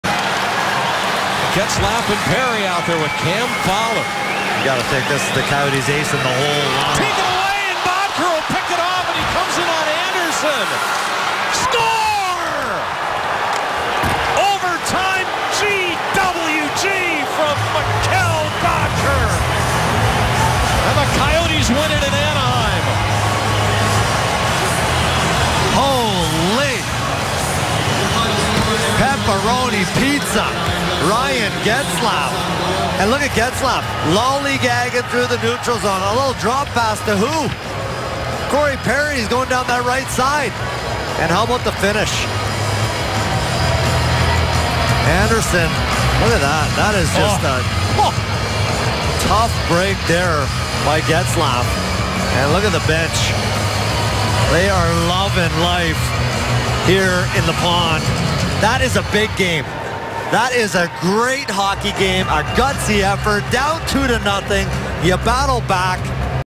Boedker dekes out Andersen for OT winner
Coyotes forward Mikkel Boedker picks up an errant drop pass from Ducks captain Ryan Getzlaf and dances around goalie Frederik Andersen for the OT winner.